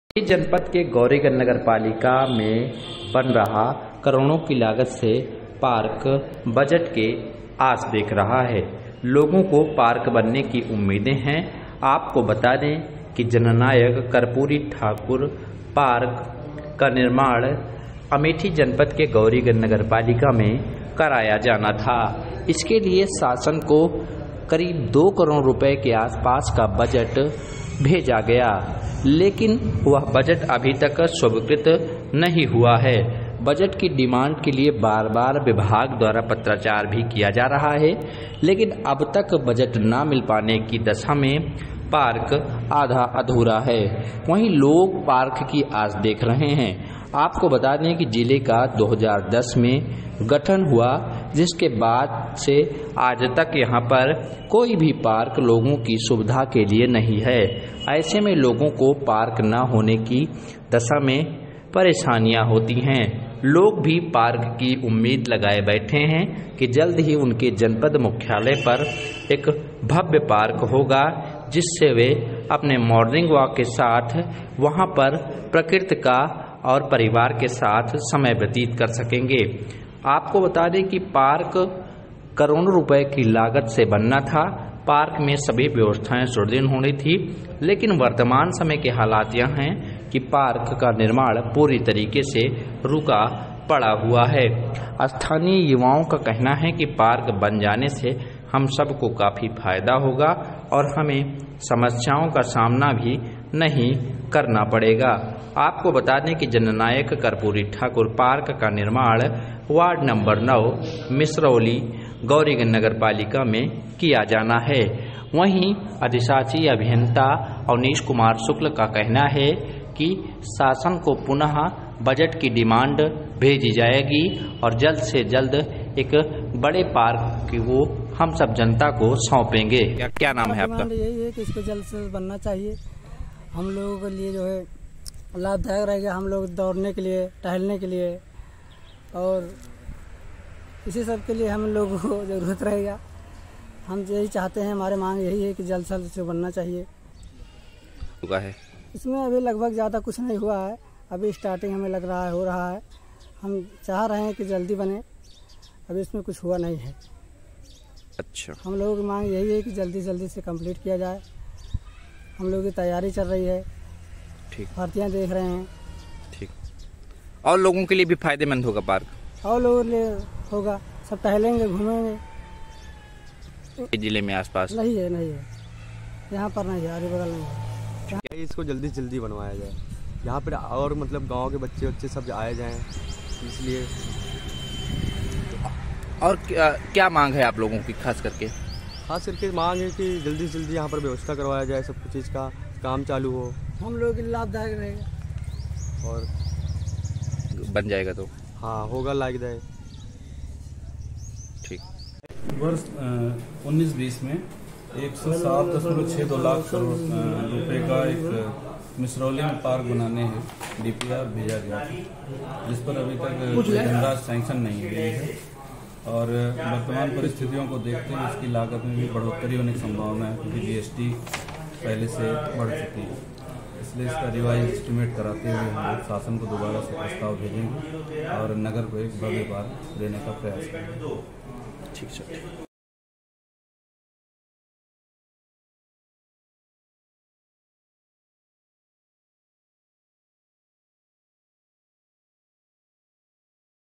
Amethi: बजट के अभाव में अधूरा पड़ा जननायक कर्पूरी ठाकुर पार्क, प्रशासन के पहल से फिर जागी उम्मीद
पार्क बन जाने से लोग पर्यावरण का आनंद ले सकेंगे. इसके साथ ही हर व्यक्ति के लिए यह पार्क फायदेमंद होगा. नगर पालिका प्रशासन द्वारा यह कहा जा रहा है कि जल्द से जल्द पार्क तैयार करवा दिया जाएगा.